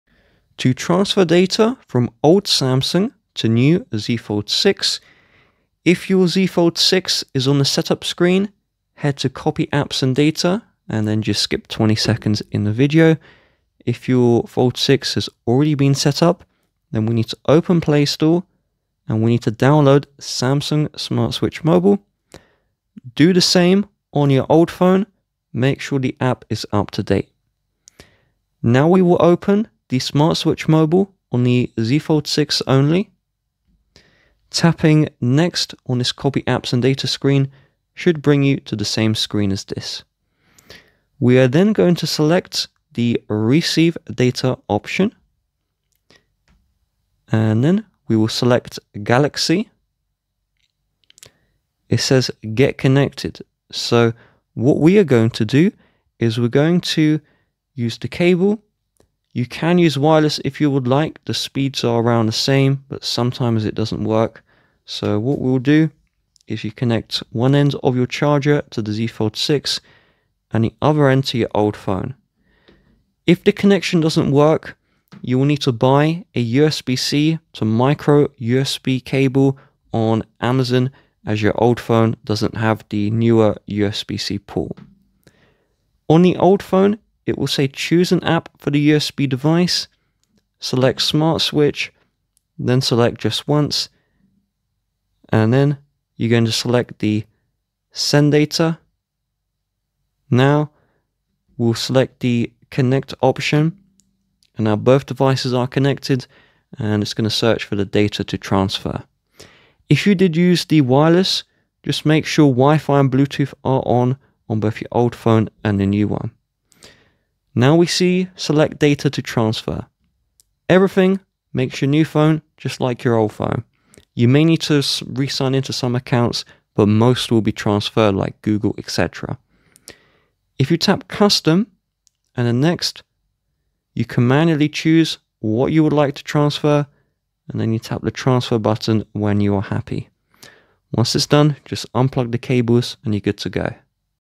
How To Transfer Data from Samsung to Samsung Galaxy Z Fold 6
Welcome to our guide on how to transfer data from a Samsung device to your new Samsung Galaxy Z Fold 6! In this video, we’ll walk you through the step-by-step process to ensure all your important data, including contacts, photos, apps, and settings, are seamlessly transferred to your Galaxy Z Fold 6. Follow along to learn how to make the switch effortlessly and get your new device set up with all your essential data.